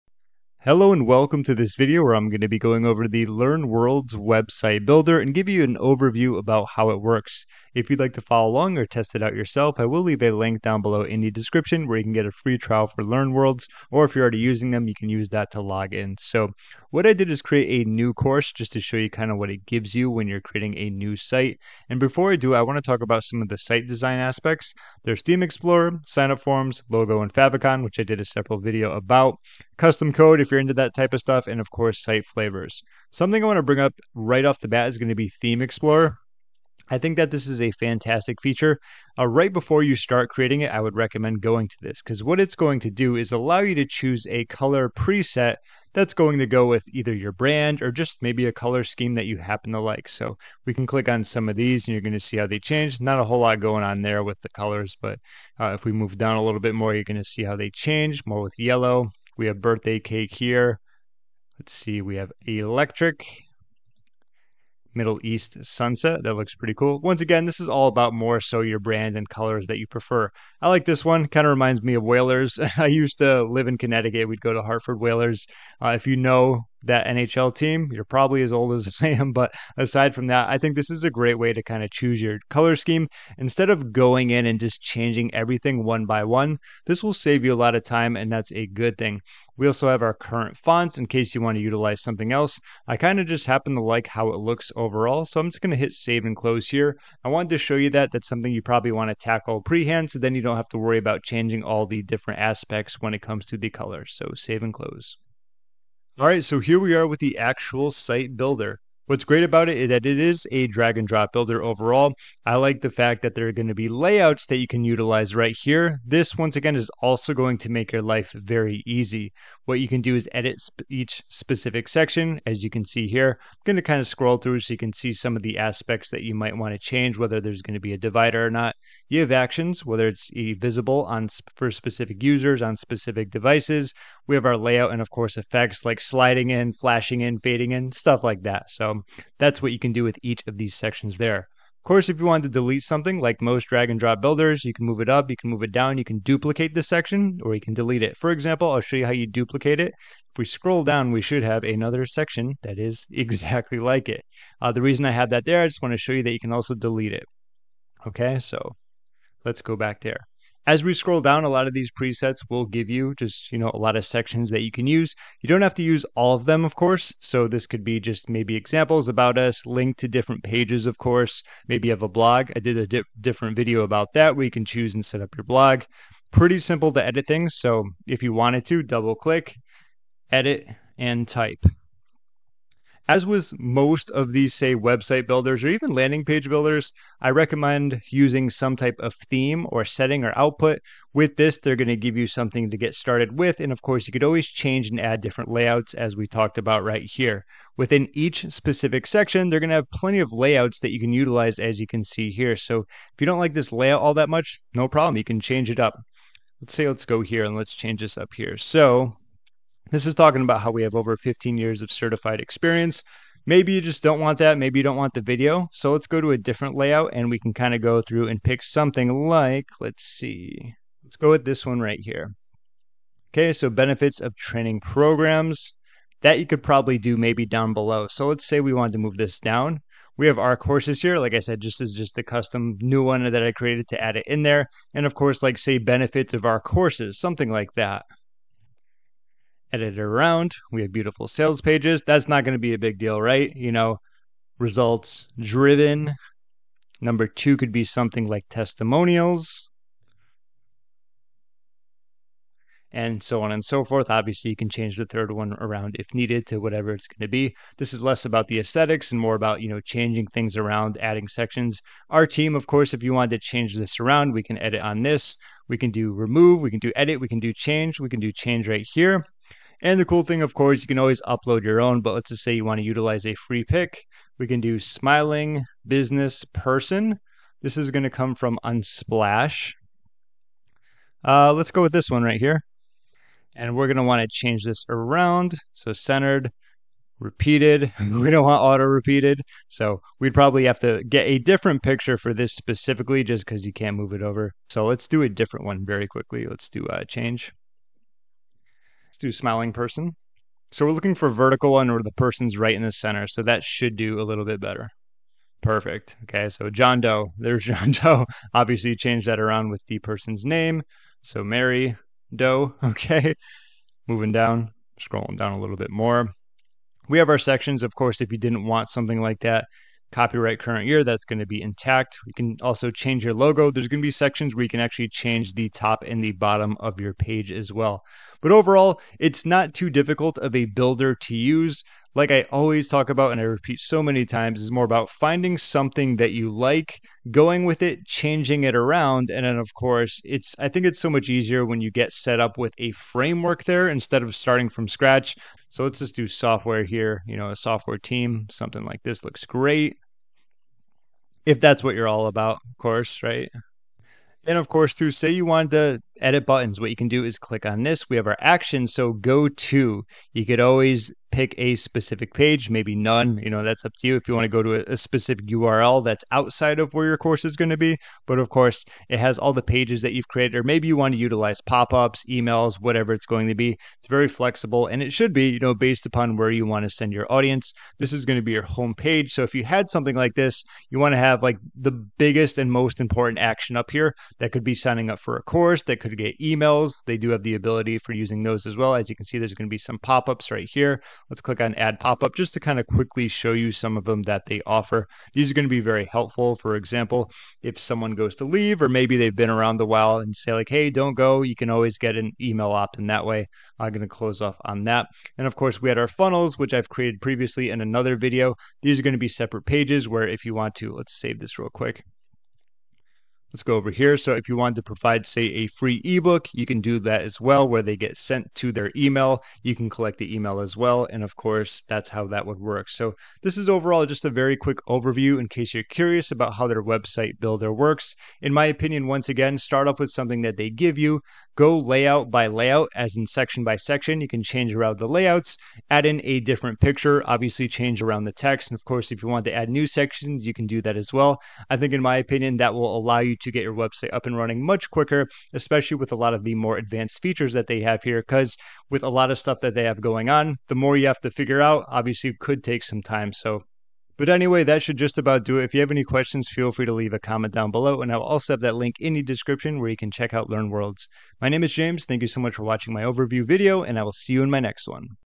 LearnWorlds Website Builder Overview (How It Works & Features)
LearnWorlds Site Builder Video

In this video, I am going to be giving you an overview of the LearnWorlds website builder. I really enjoy the fact that they come with a theme explorer, which allows you to set the color theme for all of your pages. This saves you time from having to change the color of every button and text box. I'll also show you how to edit and change around the text, pictures, and various settings within the website builder. This should give you a good idea about how their website builder works.